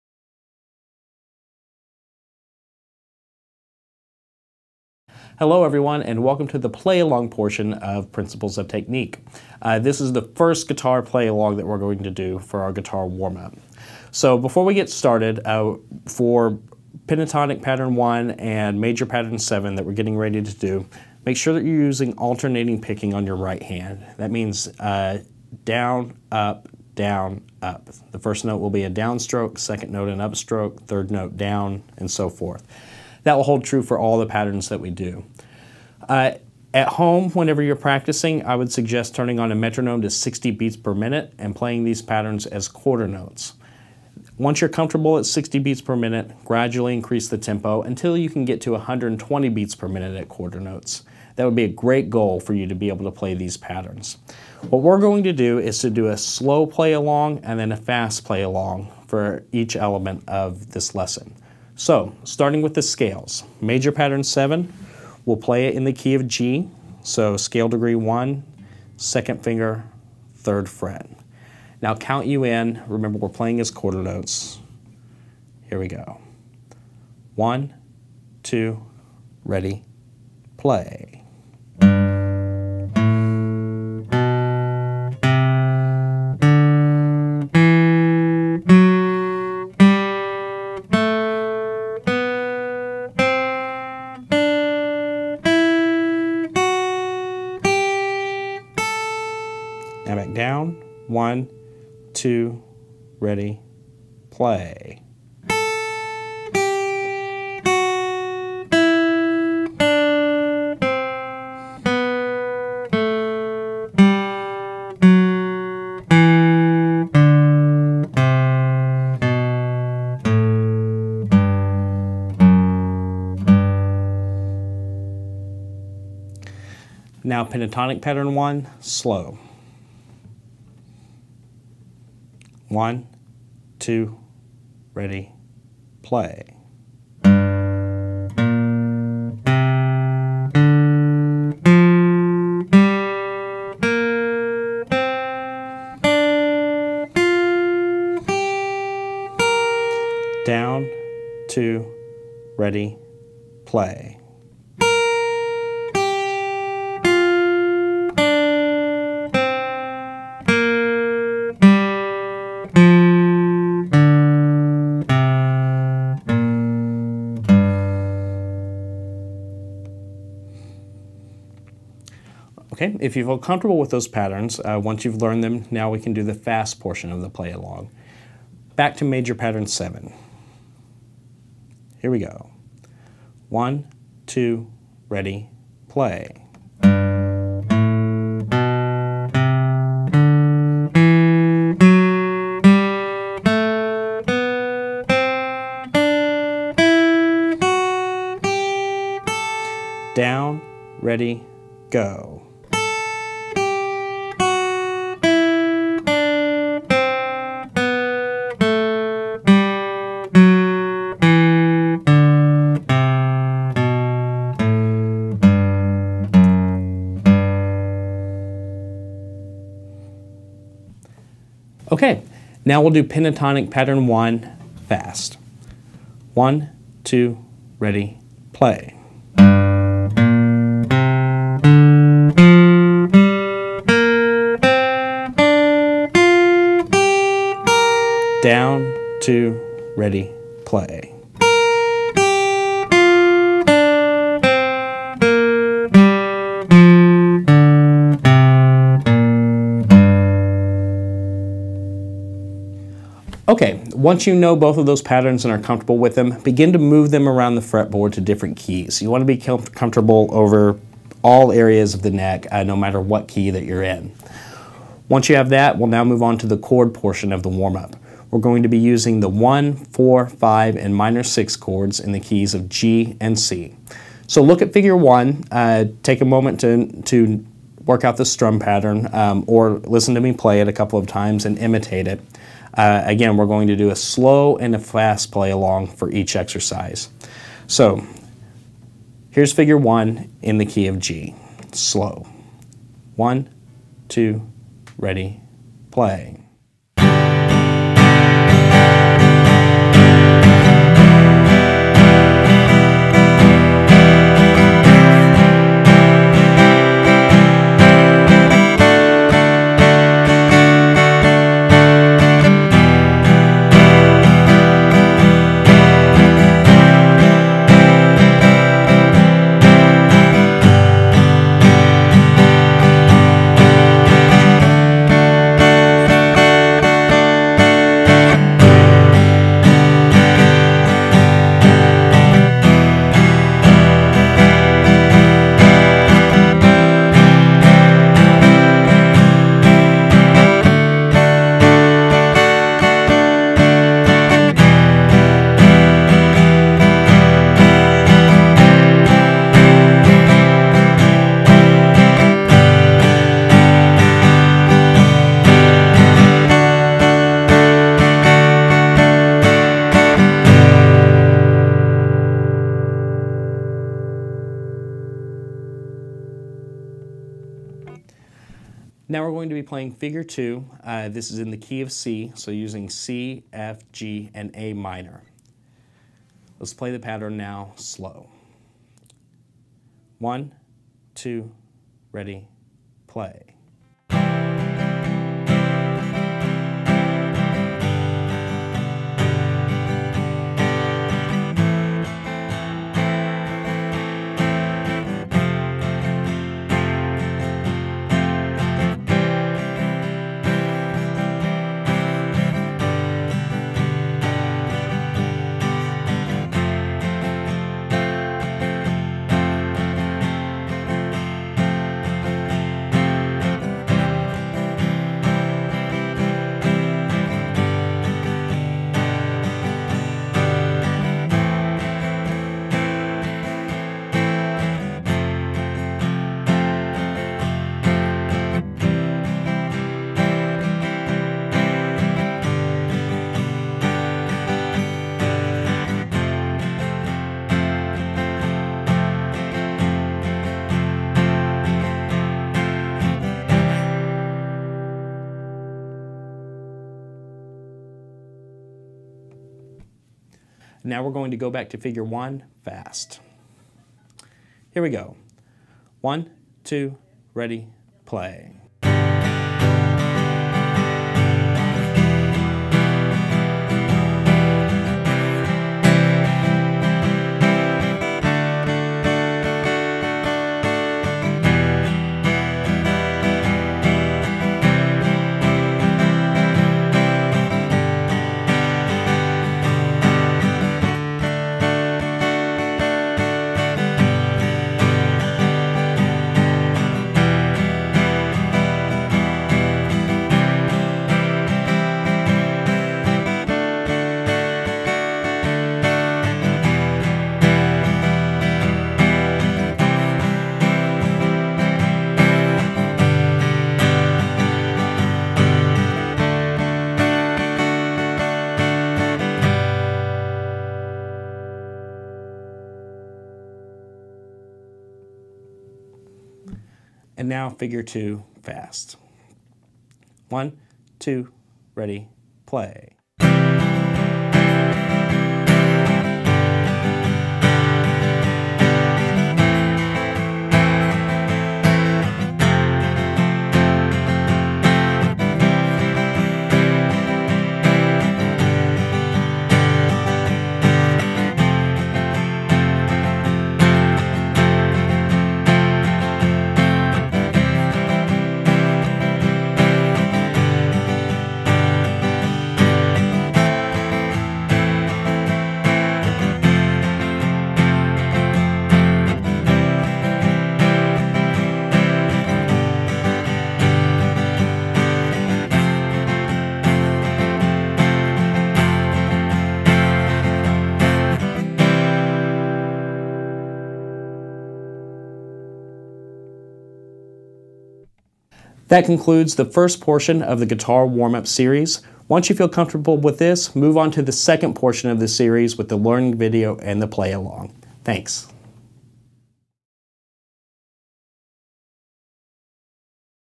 Principles of Technique: Guitar Warm-Up One Tutorial
Major Pattern One - Pentatonic Pattern One - Chords in the keys of G and C.